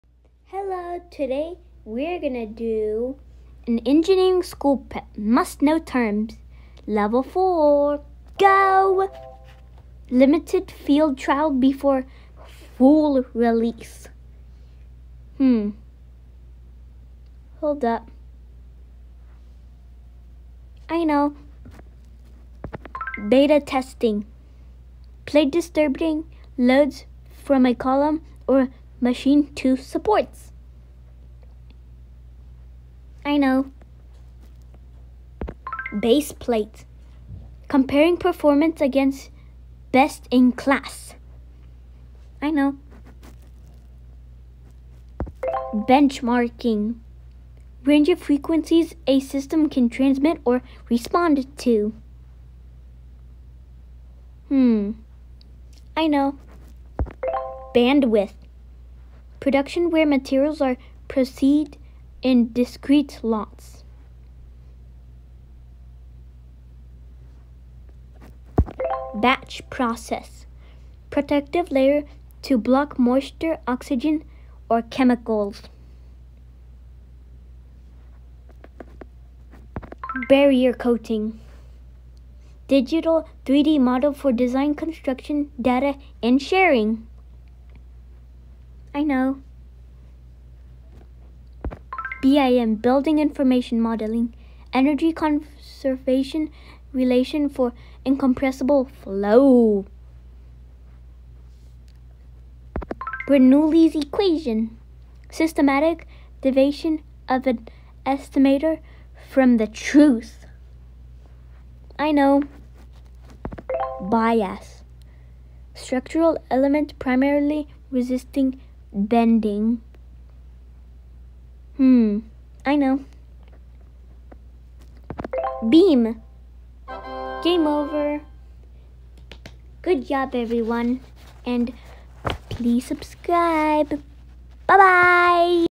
L4, Engineering School Prep 20,000+ Comprehensive Terminology Words
⚙️⭐️Comprehensive, cross-discipline core vocabulary set, broad terms used across mechanical, electrical, civil, chemical, biomedical, and more…

Acronyms, symbols, phenomena, materials, devices, processes used in engineering disciplines.
fundamentals,
Discipline-specific-
electronics, electromagnetics, thermodynamics, materials, building, construction, structural, microfluidics, nanomaterials, bioinstrumentation, and more…